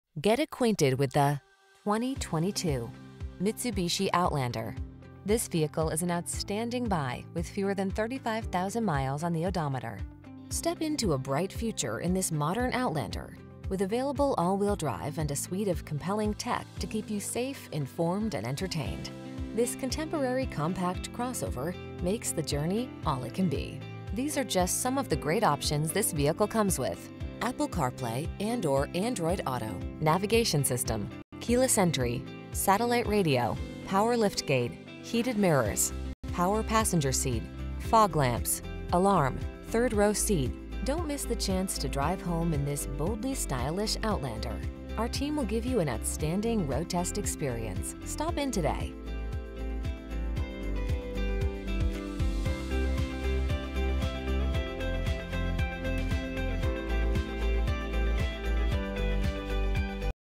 2022 Mitsubishi Outlander SEL MA Milford, Franklin, Worcester, Framingham MA Providence
Pre-Owned 2022 Mitsubishi Outlander SEL available at Imperial Chevrolet located in 18 Uxbridge Rd, Mendon, MA, 01756. Servicing the Milford, Franklin, Worcester, Framingham MA Providence area.

Get acquainted with the  2022  Mitsubishi Outlander.  Step into a bright future in this modern Outlander. With available all-wheel-drive and a suite of compelling tech to keep you safe, informed, and entertained, this contemporary, compact crossover makes the journey all it can be. These are just some of the great options this vehicle comes with: Apple Carplay®/Android Auto®, Navigation System, Keyless Entry, Satellite Radio, Power Liftgate, Heated Mirrors, Power Passenger Seat, Fog Lamps, Alarm, 3rd Row Seat. Don't miss the chance to drive home in this boldly stylish Outlander. Our team will give you an outstanding road test experience. Stop in today!